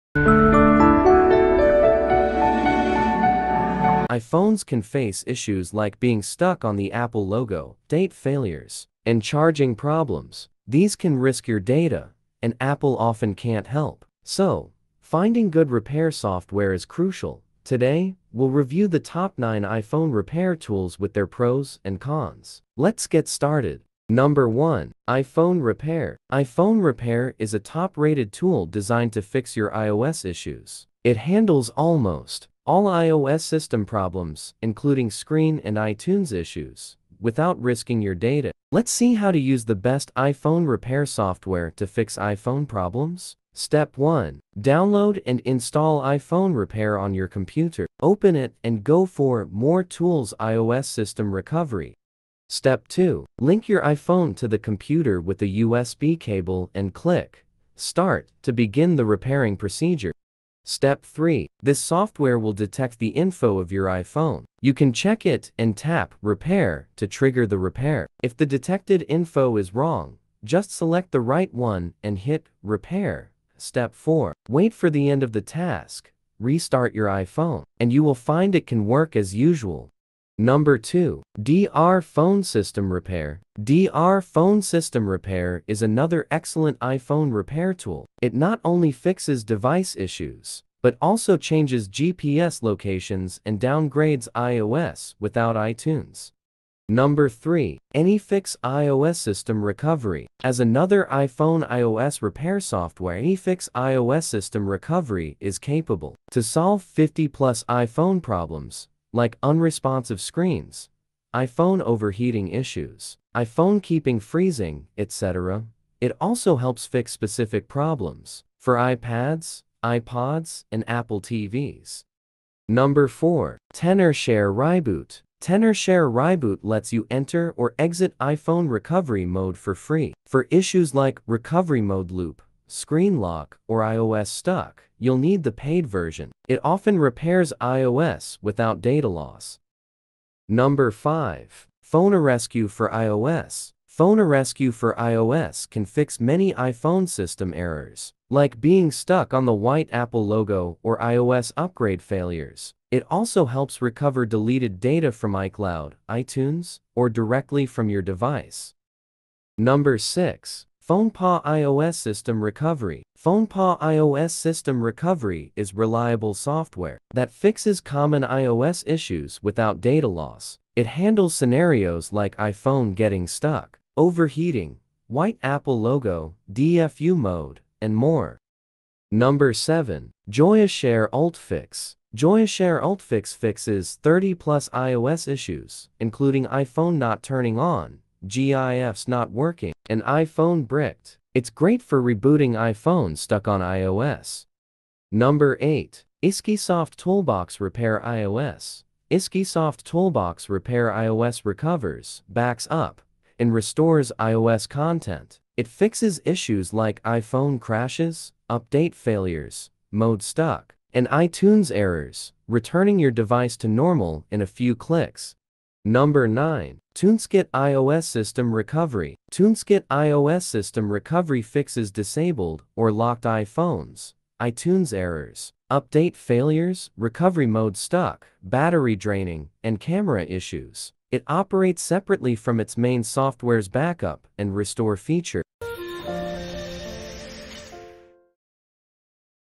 9 Best iPhone Repair Software to Fix Various iPhone Software Issues
This review lists the 9 best iPhone repair software. If you encounter an iPhone problem like black screen of death, iOS upgrade failure, etc., you can choose any iPhone repair tool to fix the issue.

00:00 Start
00:23 No.1. iPhone Repair
01:20 No.2. Dr.Fone - System Repair
01:36 No.3. AnyFix – iOS System Recovery
02:02 No.4. Tenorshare – ReiBoot
02:20 No.5. PhoneRescue for iOS
02:38 No.6. FonePaw iOS System Recovery
02:58 No.7. Joyoshare UltFix
03:14 No.8. iSkysoft Toolbox – Repair (iOS)
03:35 No.9. Tuneskit iOS System Recovery